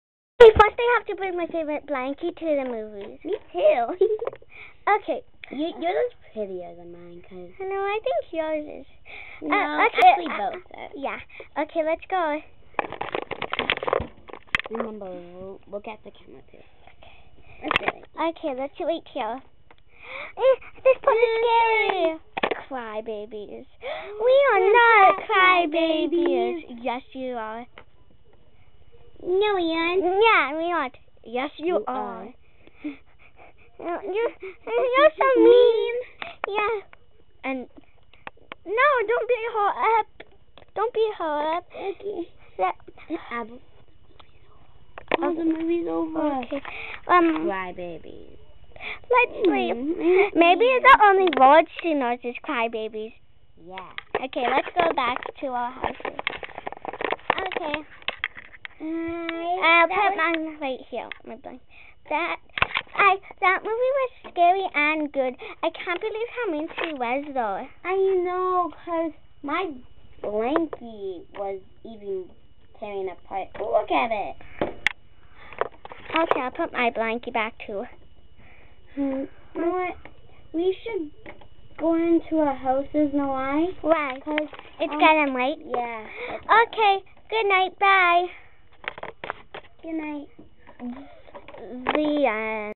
the piggy friends (rockCandN made this with me) part 2
they go tro the movie's and it's realy scary a mean bunny call's the cry baby's end off the piggy friends video's